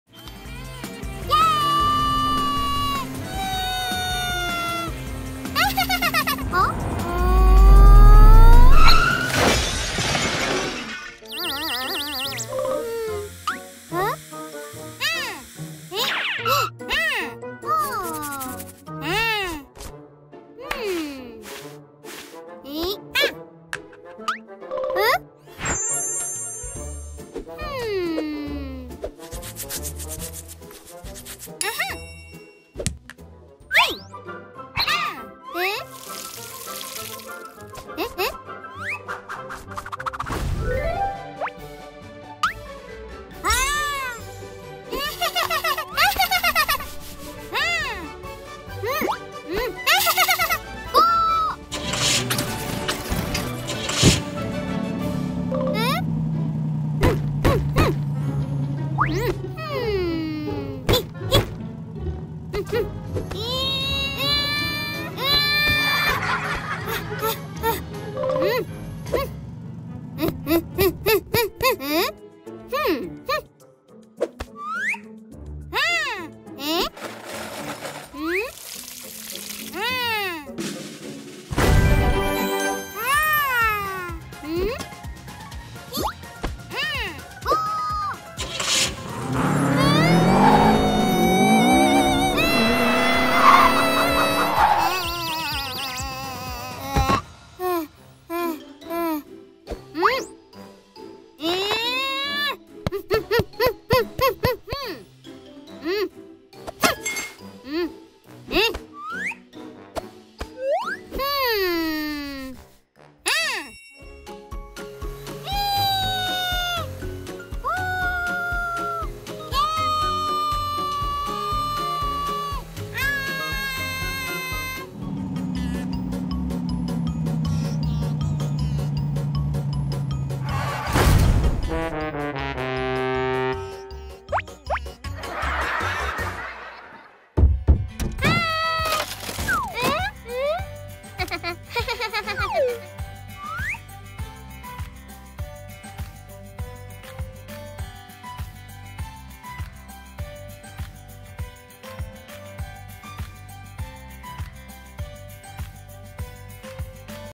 Unlocking The Colorful World: Magic Cars And Kids' Captivation| Kitta Wonderland
Get ready for your kids to be amazed and entertained with these magical and colorful cars! These powerfully colorful cars will capture your children's imagination and provide endless hours of fun. Watch as your kids fall in love with these magical vehicles and their vibrant colors. Perfect for children of all ages!
What wonders are hidden in these colorful cars? Let's explore the exciting surprises of toy car colors with Kitta in the animated video below.